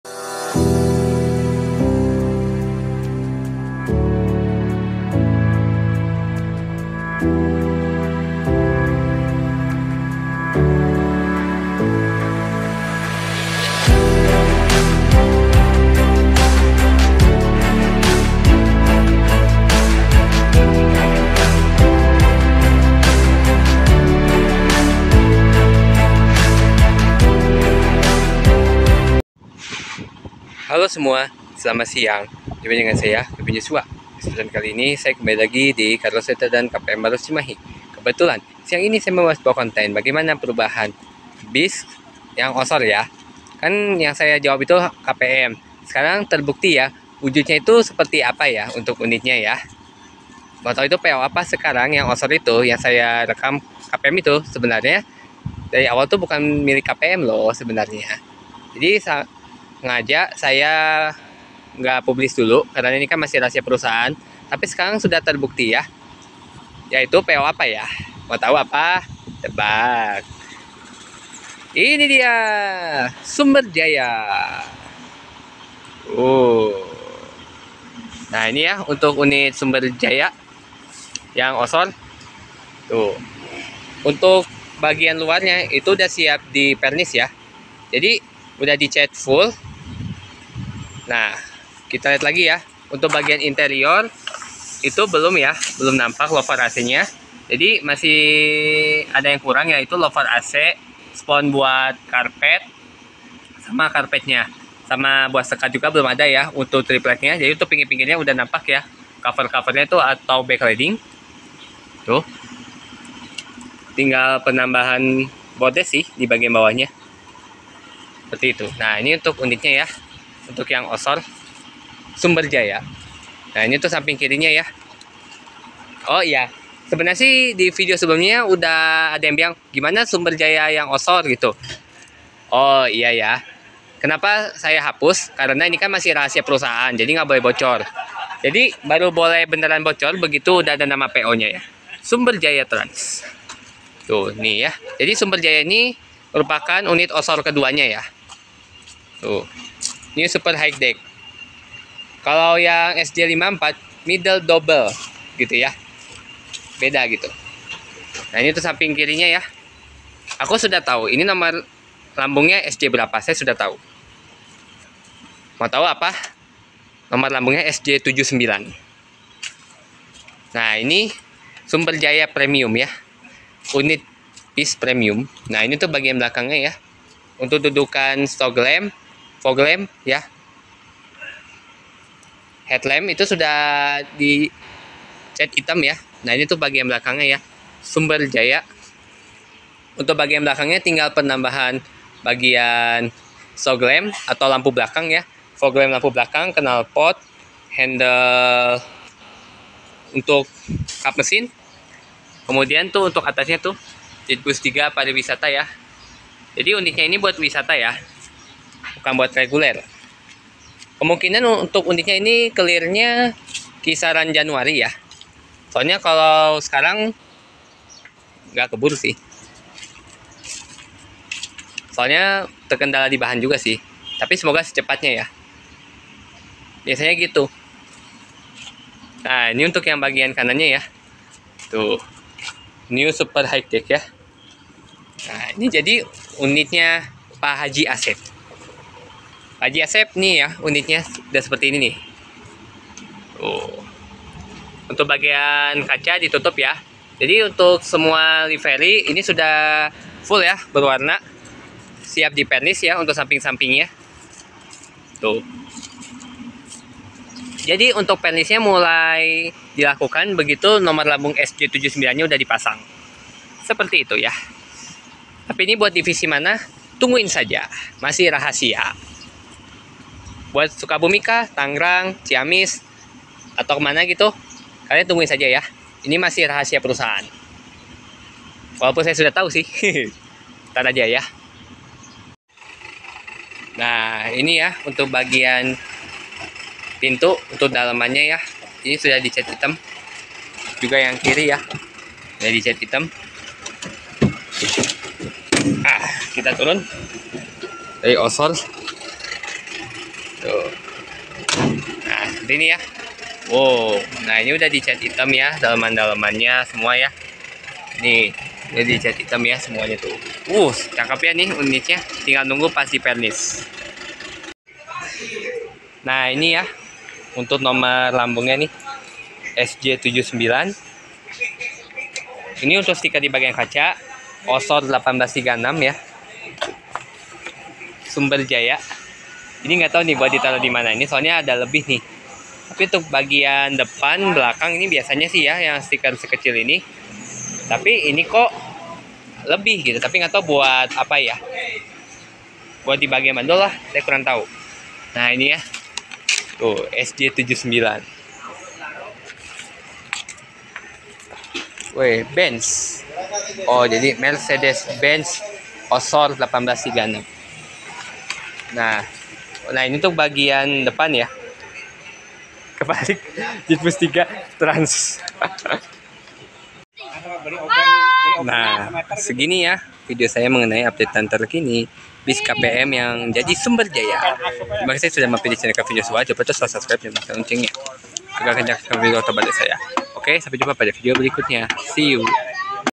TERJAWAB SUDAH, PUNYA PO MANAKAH UNIT BUS KPM 0500R INI. SEGIMANA TAHAP PENGERJAAN BAGIAN LUARNYA
hello lur, kali ini rahasia kita sudah dipublish, mau tau PO manakah?